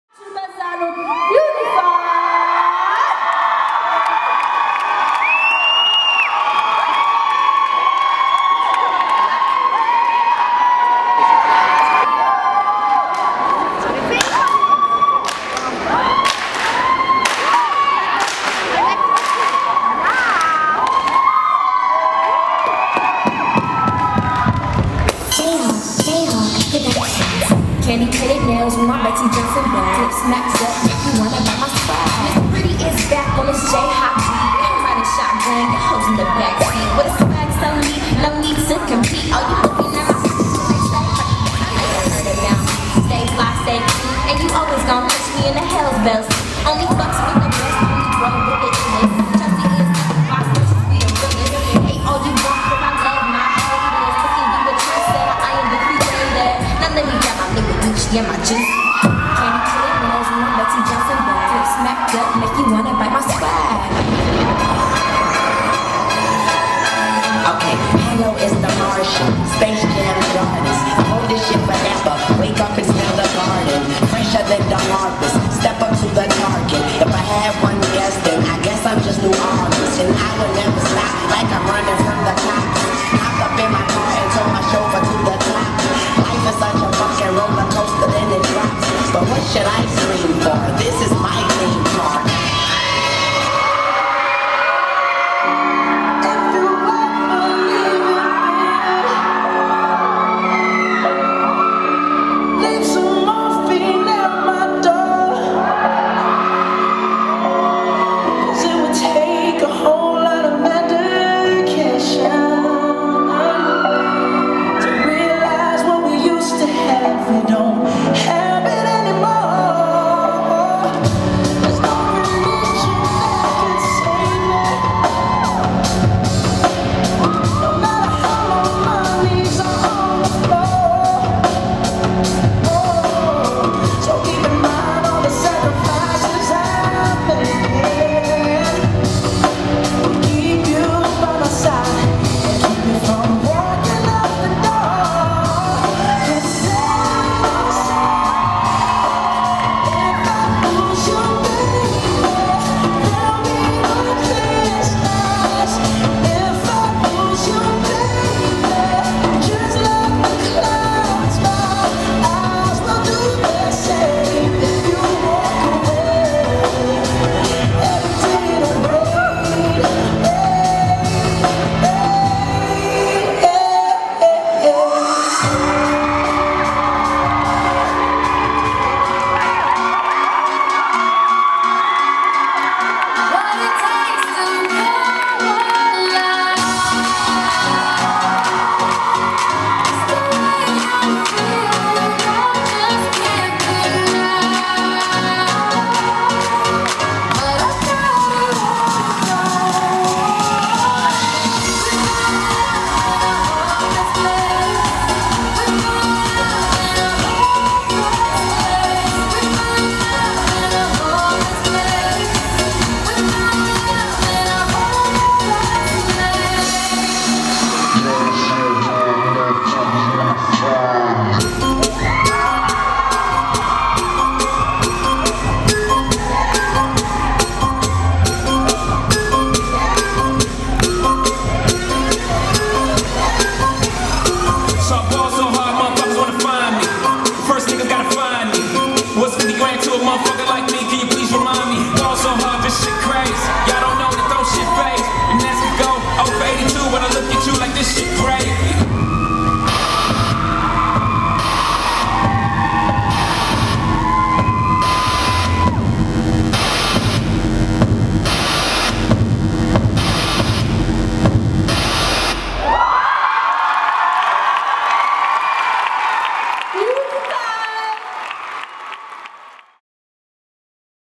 D!´s Dance Competition 2012 - UNIFY- Deutsche Meisterschaft - I&A Teens
Unify ein MIX aus Liquid Beatz , Random & Alpha Animals belegten auf der Deutschen Meisterschaft (D!´s Dance Competition 2012) den 1. Platz